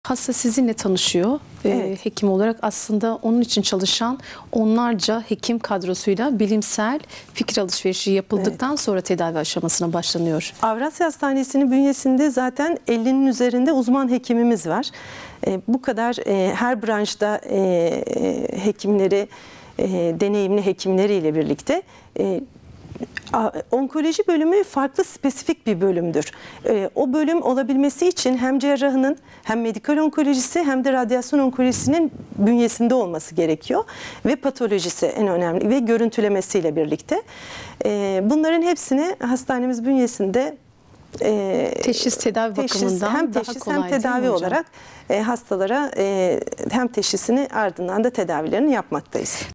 Kanser Tedavisinde Avrasya Hospital ?
Kanser Tedavisinde Avrasya Hospital ? Avrasya Hospital Radyasyon Onkolojisi Uzmanı Uz.Dr. Züleyha KADEHÇİ Bilgilendiriyor.
Avrasya Hospital Sağlıklı Hayat Programı